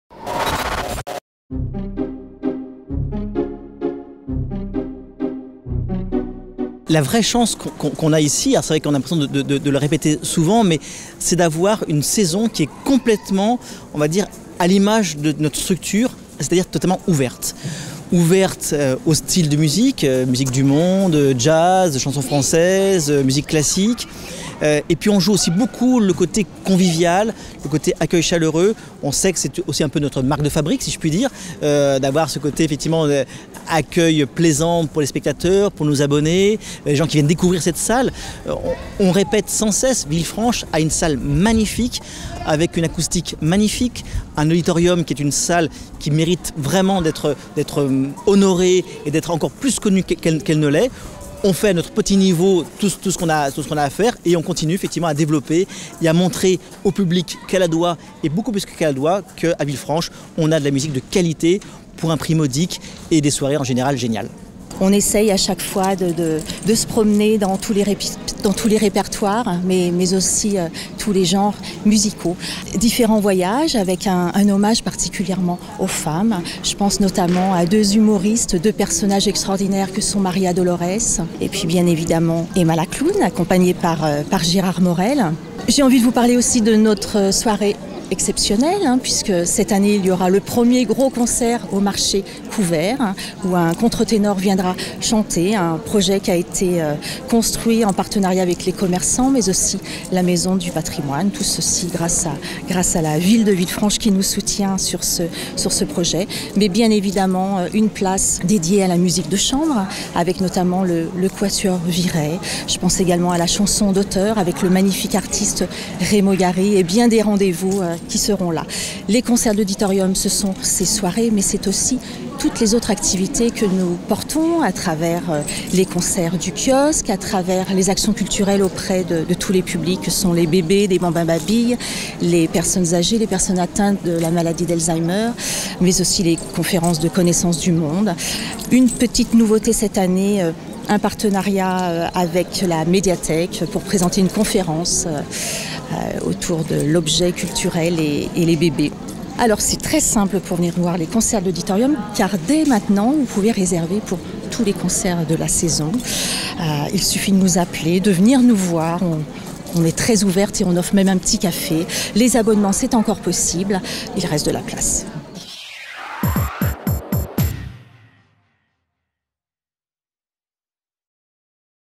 Concerts de L'Auditorium Villefranche - Saison 2018 2019
C'est parti pour la saison 2018/2019 des Concerts de l'Auditorium de Villefranche-sur-Saône. Découvrez cette 23ème saison avec Olivier Charrin, le président de l'association et Cécile Jacquemont, sa directrice artistique.